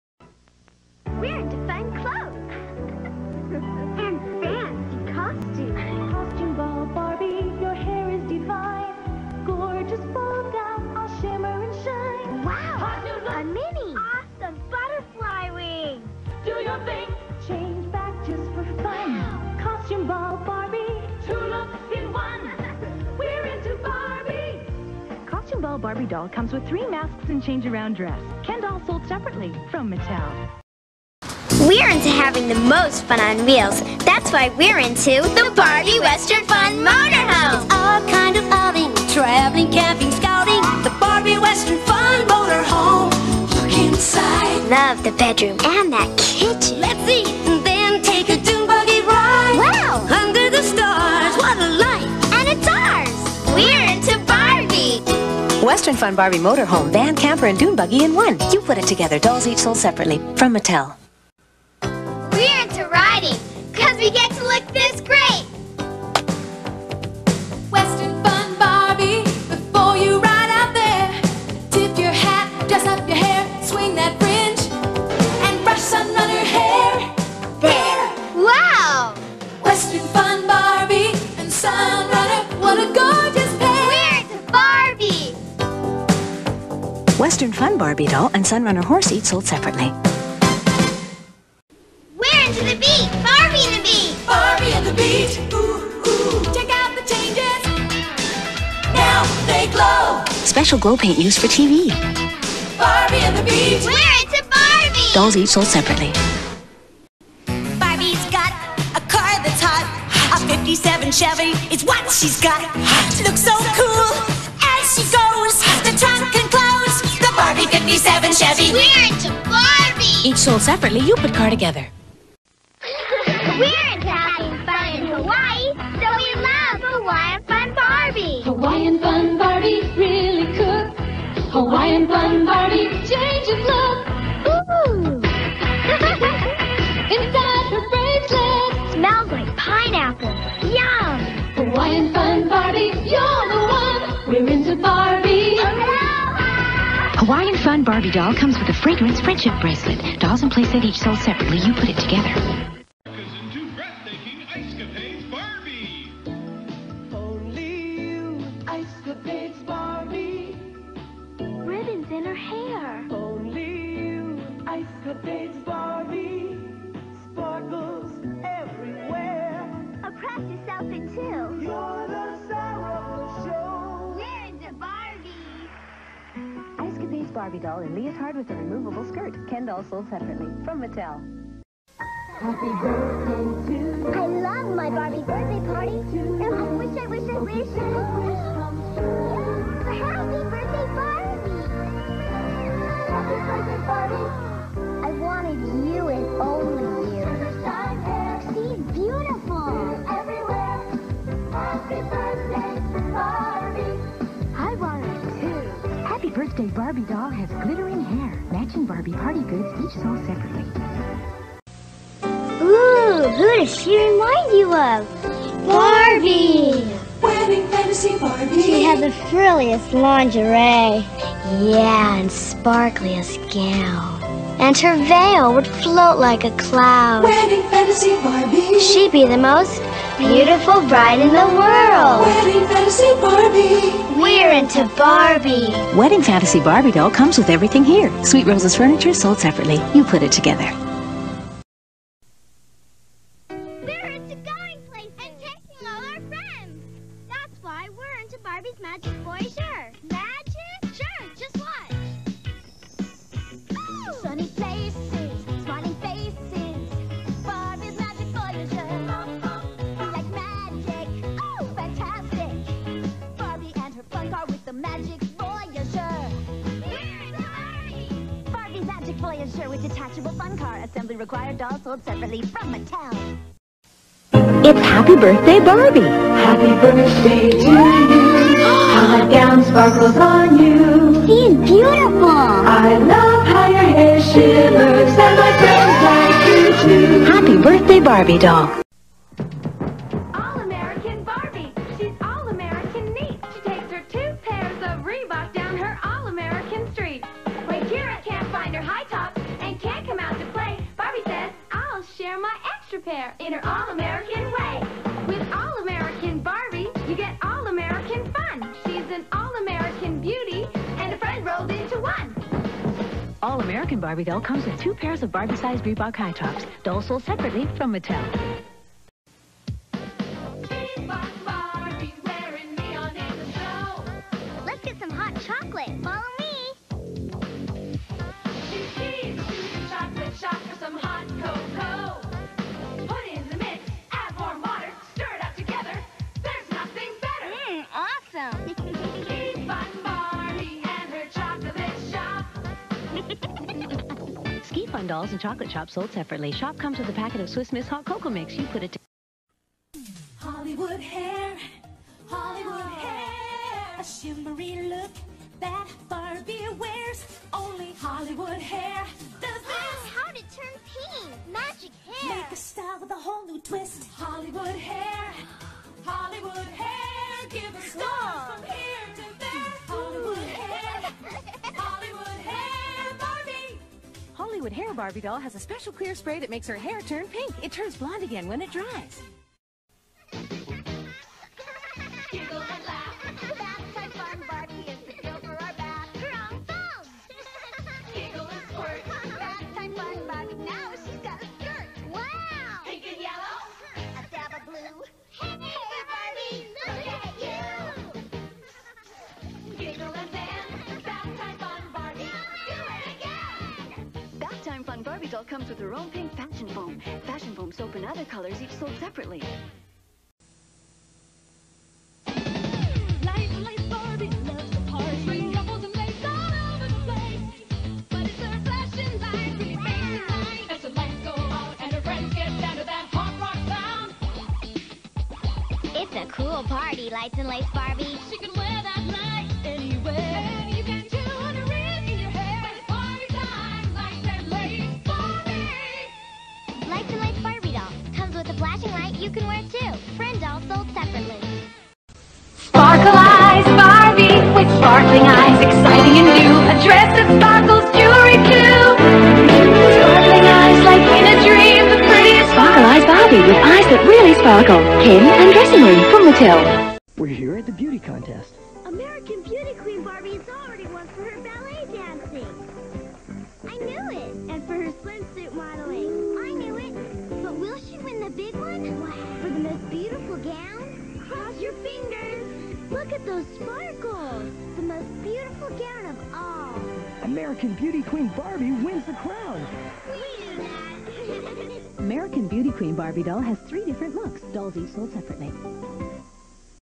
Barbie commercial compilation 1990 1991
And now for all you women out there that remembers Barbie, here is a compilation of Barbie commercials from 1990 and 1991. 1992 to 1994 will be coming up soon. I don't know when but it will come. All rights to all Barbie commercials go to Mattel inc. No Infringement intended.